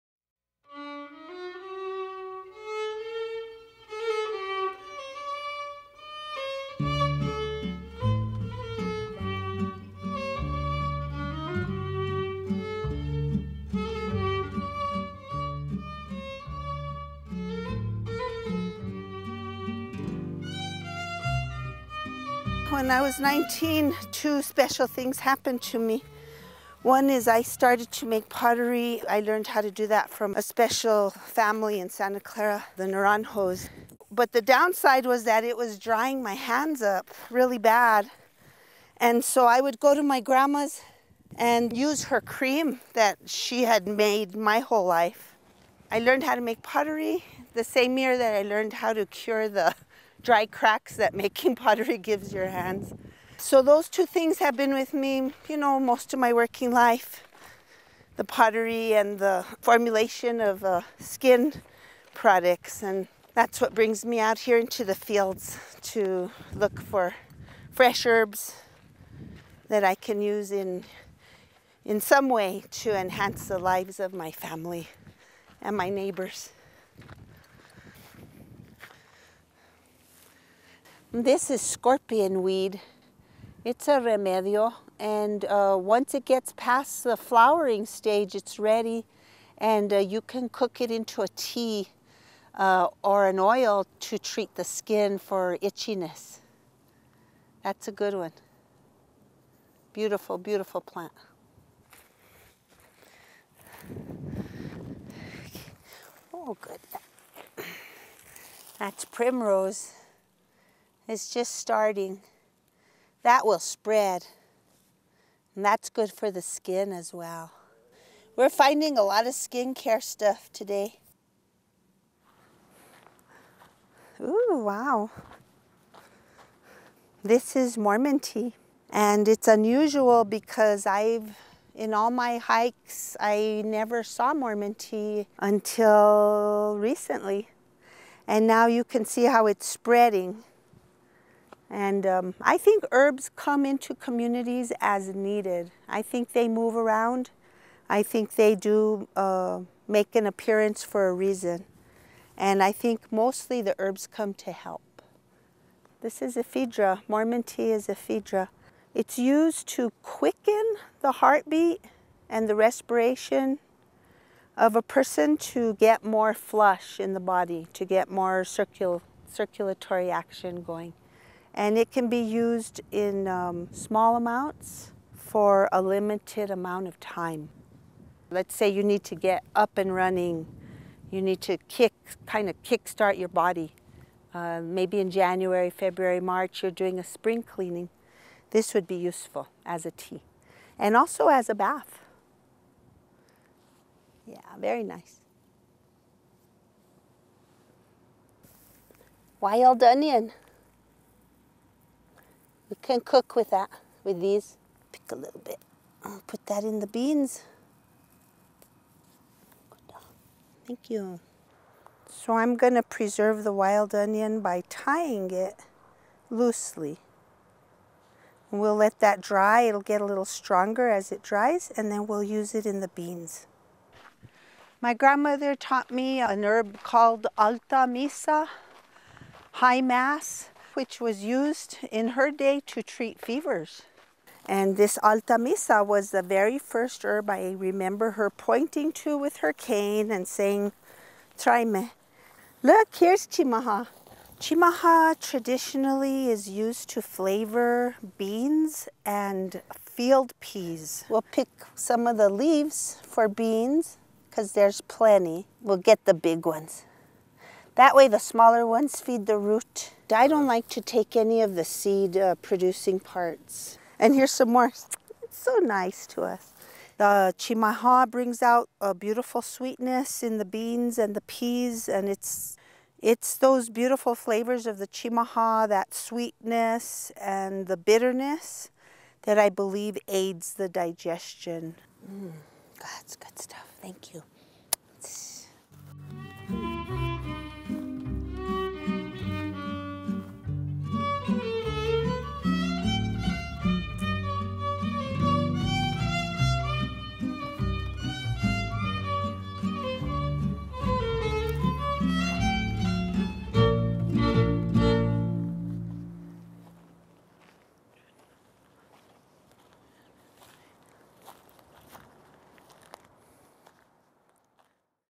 New Mexican Spring Herb Walk with "Medica" Camilla Trujillo
Follow Camilla Trujillo as she searches for wild spring herbs to use in her traditional medicamentos and recipes of Northern New Mexican cuisine.  She finds scorpion weed, primrose, mormon tea, wild onion and chimaja, an ancient herb used in the preparation of beans.